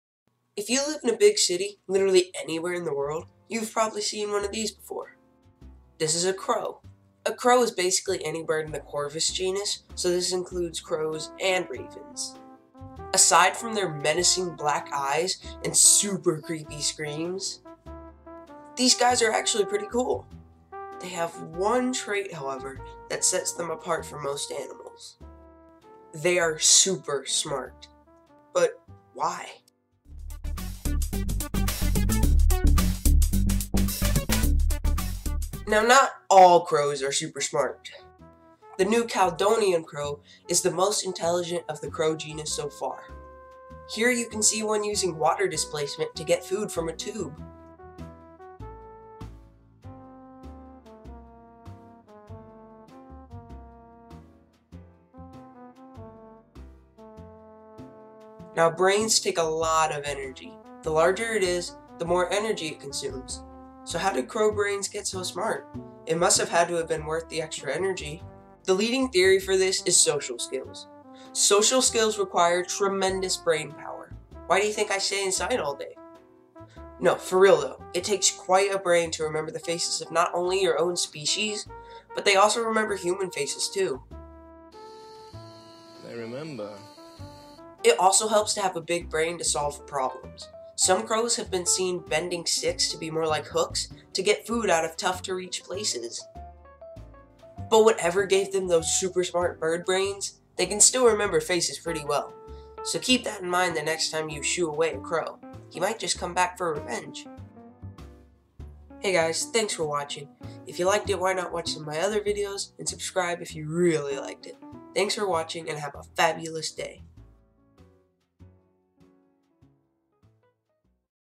Why Are Crows So Smart?
If you live in a big city literally anywhere in the world, you probably have seen one of these before.

This is a crow. A crow is basically any bird in the Corvus genus. So this includes crows and ravens.

Aside from their menacing black eyes and super creepy screams, these guys are actually pretty cool.

They have one trait however that sets them apart from most animals... They are super smart. But why?

Now not all crows are super smart. The New Caledonian crow is most intelligent of the crow genus so far. Here you can see one using water displacement to get food from a tube:

Now brains take a lot of energy. The larger it is, the more energy it consumes. So how did crow brains get so smart? It must have had to have been worth the extra energy.

The leading theory for this is social skills. Social skills require tremendous brain power. Why do you think I stay inside all day? For real, though, it takes quite a brain to remember the faces of not only your own species, but they also remember human faces too.

It also helps to have a big brain to solve problems. Some crows have been seen bending stick to be more like hooks to get food from out of tough to reach places.

But whatever gave them those super smart bird brains, they can still remember faces pretty well. So keep that in mind the next time you shoo away a crow, he might just come back for revenge.

And also a documentary I saw in 7th grade. I'm going off memory here.